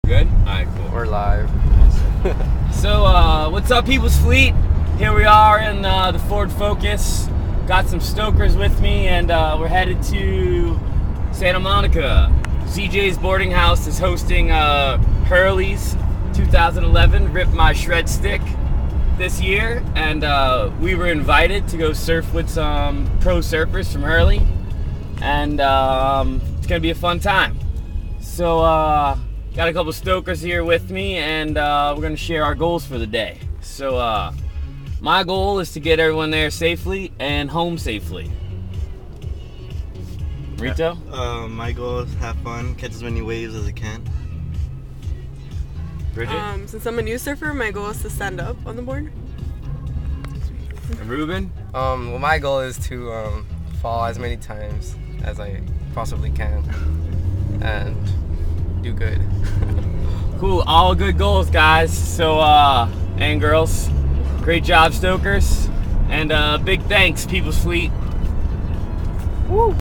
Stoked HRMSS 1.AVI
While heading to Santa Monica to surf in the Hurley, Rip My Shred Stick 2011 Competition, the Stokers share their goals for the day.